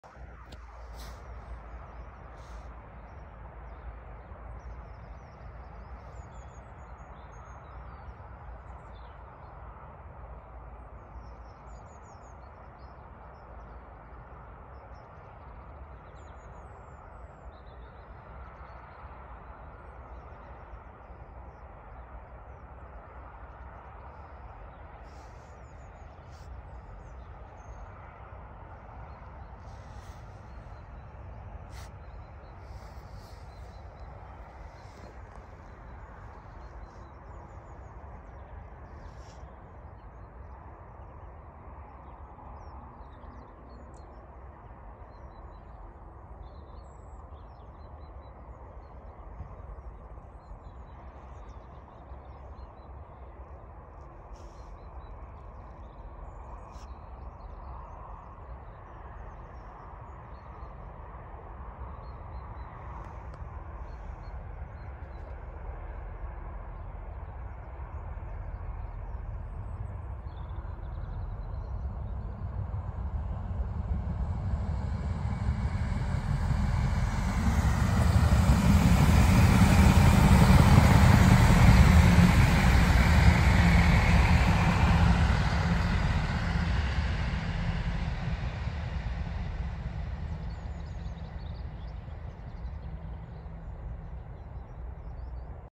The great western railway Thames turbo unit on the ECS movement to Doncaster 14/01/2024
Our Sundays train spotting video takes me to the fields just within stoke priors

On a brilliant sunny morning

And todays train you’re looking at is the 5Q23 and the really early 08:13AM service to Doncaster works

And for the Second time in succession

I get to see a 3 car class 166 Thames turbo unit

These units which can still be seen operating services in and around Bristol and the south west

So here’s the unit crawling slowly for a signal check waiting for a train from Hereford to come and take the line ahead